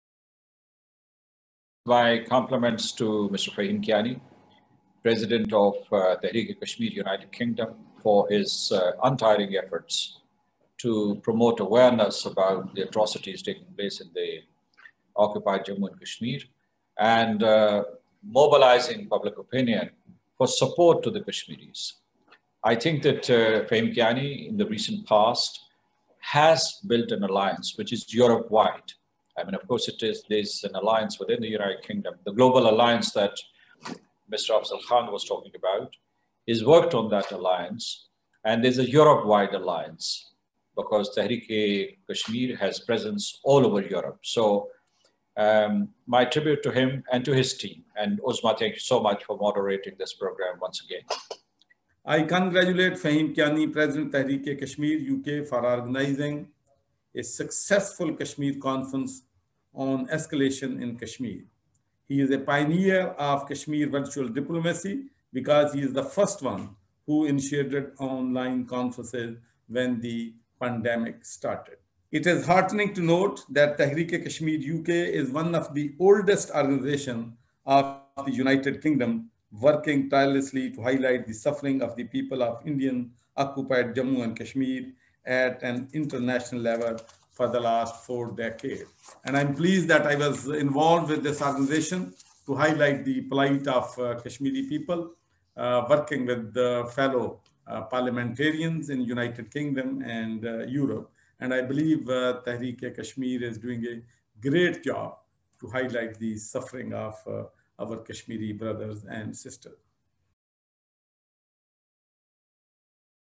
Kashmir Virtual Diplomacy, Sardar Masood Khan President AJK,Ch M Sarwar Gov. of Punjab, Fahim Kayani
Sardar Masood Khan President of Azad Jammu and Kashmir ,Ch Muhammad Sarwar Governor of Punjab commended the efforts of Fahim Kayani President Tehreek-e-Kashmir UK at an international level . They said that Fahim Kayani President e Kashmir UK is a poineer of Kashmir Virtual Diplomacy and he built alliances across  Europe to gather support on the Kashmir issue